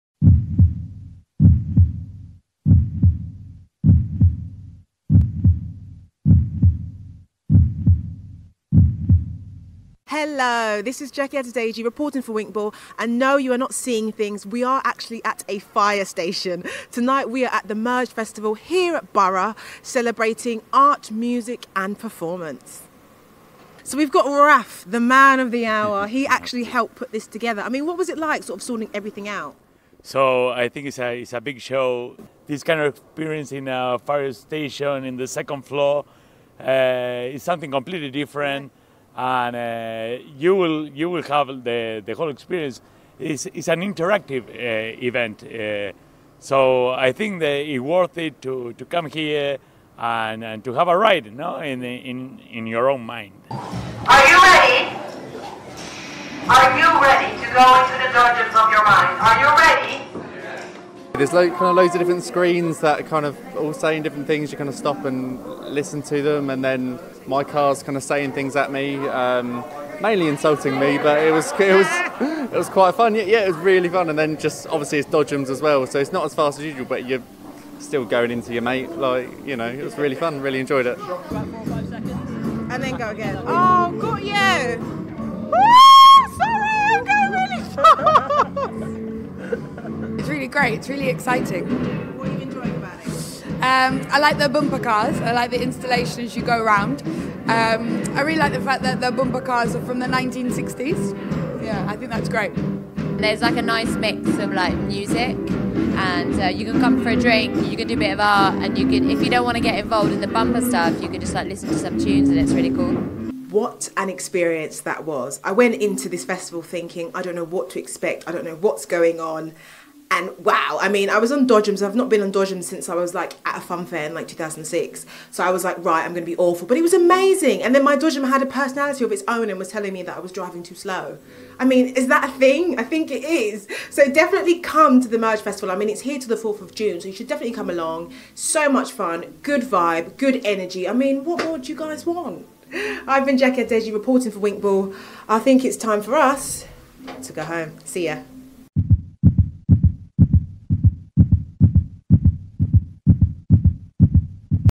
Drive Dead Slow: Dodgems of Your Mind by WinkBall
Take a psychological journey in a 1960's Dodgem Car at a disused fire station in London SE1 'Dodgems of your mind' is the world's first artist-designed bumper car arena. for more interviews